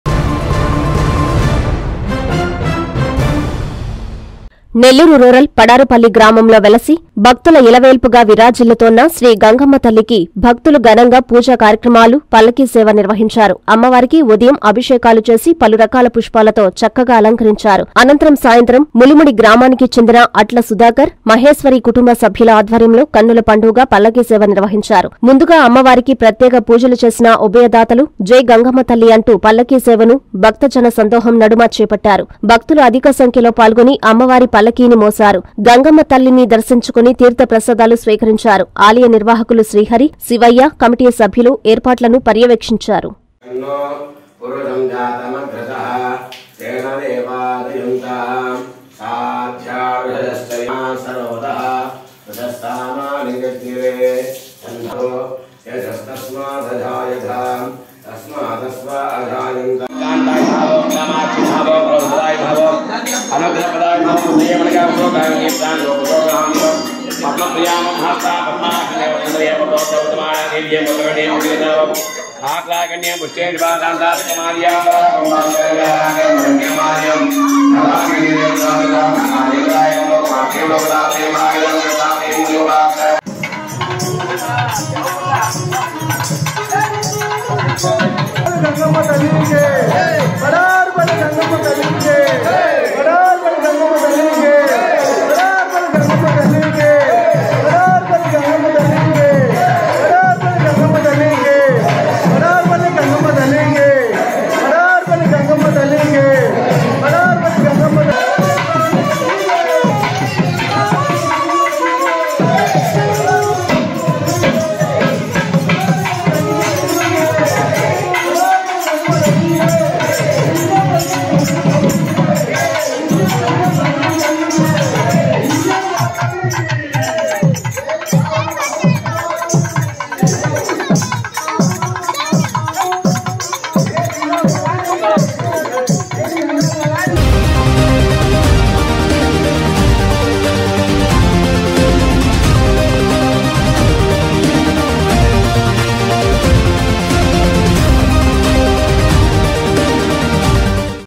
ప‌ల్ల‌కీపై ఊరేగిన గంగ‌మ్మ‌త‌ల్లి | Pallaki Seva | Gangammathali
- అమ్మ‌వారిని ద‌ర్శించుకుని తీర్థ‌ప్ర‌సాదాలు స్వీక‌రించిన భ‌క్తులు
- ఉభ‌య‌దాత‌లుగా ములుముడికి చెందిన అట్ల సుధాకర్, మ‌హేహేశ్వరి కుటుంబ‌స‌భ్యులు
- ఏర్పాట్ల‌ను ప‌ర్య‌వేక్షించిన ఆల‌య నిర్వాహ‌కులు శ్రీహ‌రి, శివ‌య్య‌, స‌భ్యులు